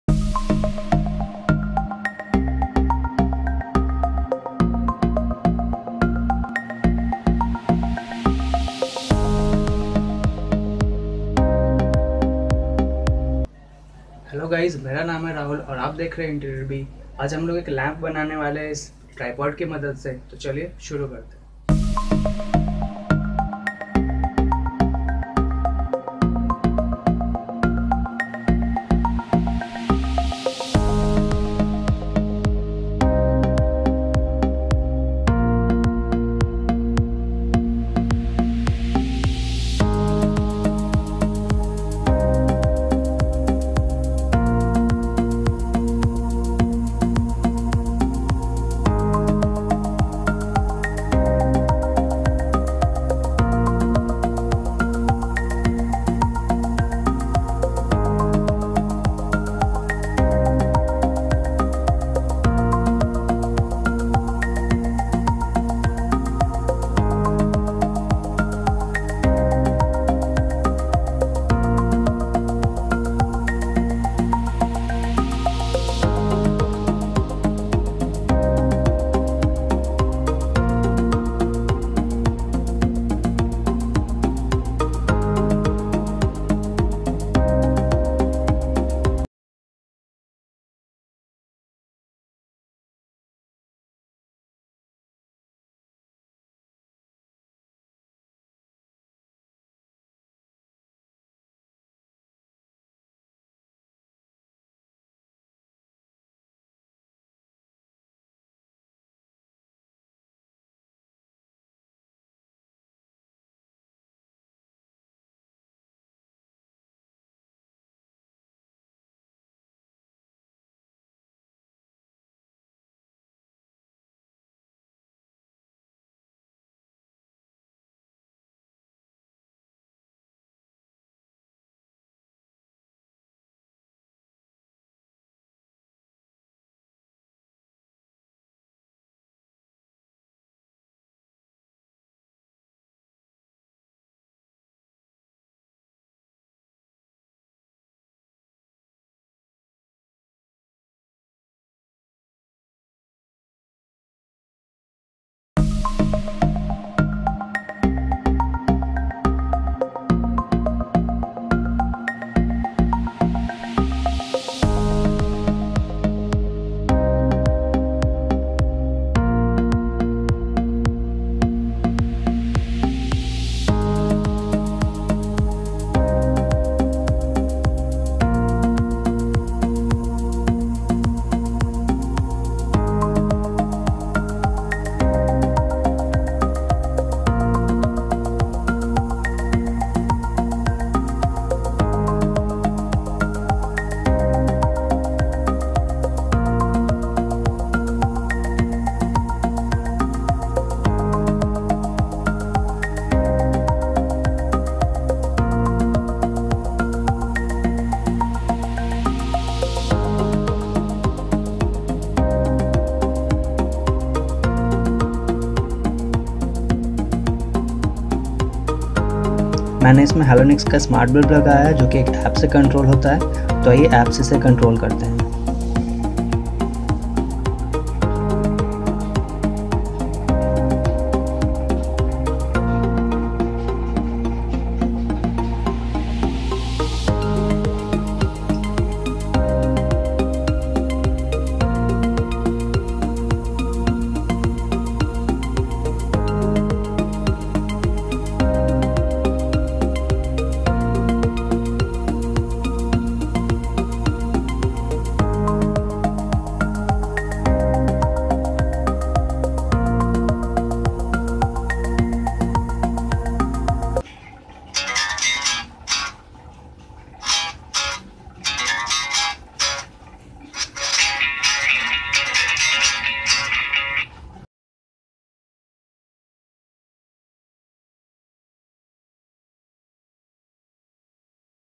DIY lamp using my tripod stand || Interior Bee
Hello guys. Made a lamp using my tripod stand and used halonix smart bulb in the lamp.